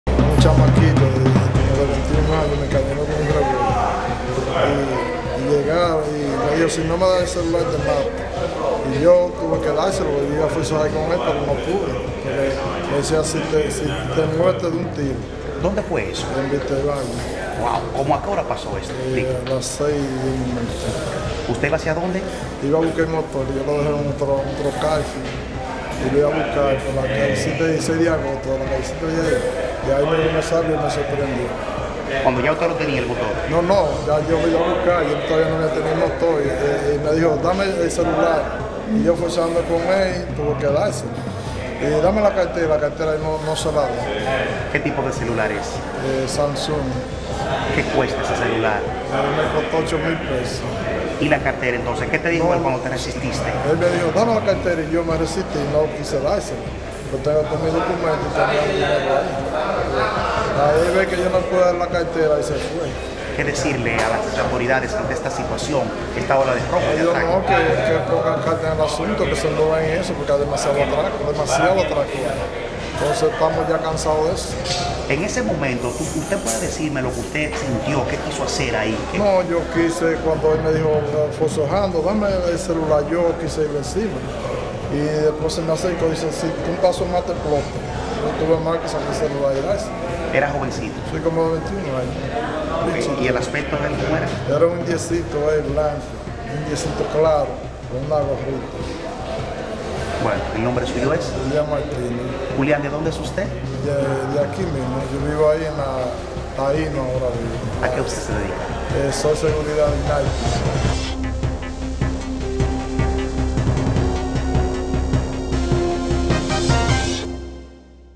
Vigilante denuncia fue atracado en Vista al Valle SFM
San Francisco de Macorís.- Un guardián de seguridad denunció que fue atracado por un joven de al menos 21 años, cuando transitaba por el sector Vista al Valle.

Julián Martínez dijo que fue interceptado en el referido sector cuando se disponía a buscar su motocicleta por un joven que lo encañonó con un revólver y le quitó un celular valorado en RD$ 8 mil.

El denunciante dijo que fue amenazado de muerte si no le daba el celular, por lo que en ese momento sintió miedo y se lo entregó.